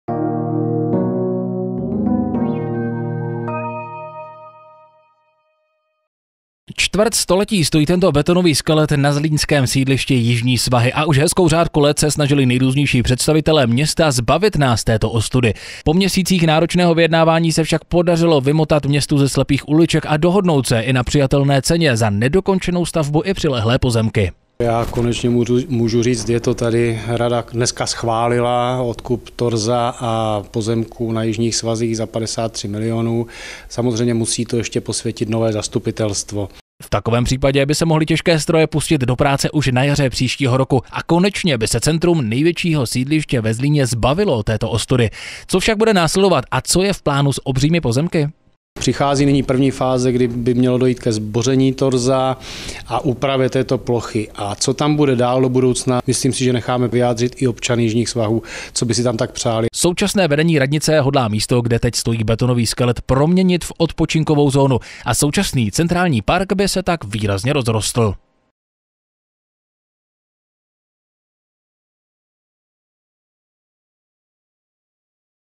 Torzo na Jižních svazích zmizí už brzy. Co vznikne místo něj?
Centrální park na Jižních svazích se podle všeho už na jaře zbaví rozestavěného obchodního centra. Konečně!  Takzvané „torzo“ na Jižních Svazích hyzdí okolí už od roku 1988. Není tak divu, že v posledním patře rostou stromy. Místo je také nebezpečné, protože majitel nedokázal pozemek zabezpečit proti vstupu. Zlínskému magistrátu se podařilo vyřešit odkup stavby a podle všeho půjde torzo konečně k zemi. Co však vznikne na pozemcích, které mají několik tisíc metrů čtverečních?